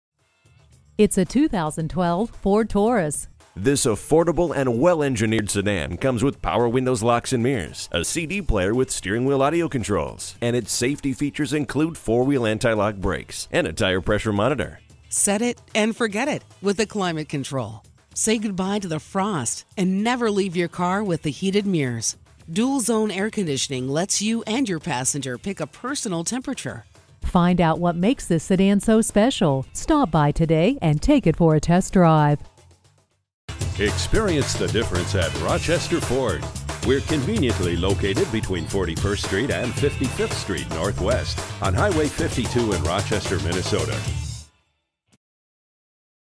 2012 Ford Taurus Rochester MN Winona, MN FA148591 - SOLD
Year: 2012
Make: Ford
Model: Taurus
Trim: SEL
Engine: Duratec 3.5L V6
Transmission: 6-Speed Automatic with Select-Shift
Color: Red
Mileage: 31446
Stock #: FA148591
VIN: 1FAHP2EW4CG135166
FWD, Bluetooth, Remote start, Satellite radio, Parking sensors. This one owner no accident recent trade has the 3.5L V6 engine, Automatic transmission, Cloth seats, Keyless entry, Aux input, Traction control, ABS brakes, CD player, Power windows and locks, Cruise control and more. We look forward to earning your business.

Address:
4900 Hwy 52 North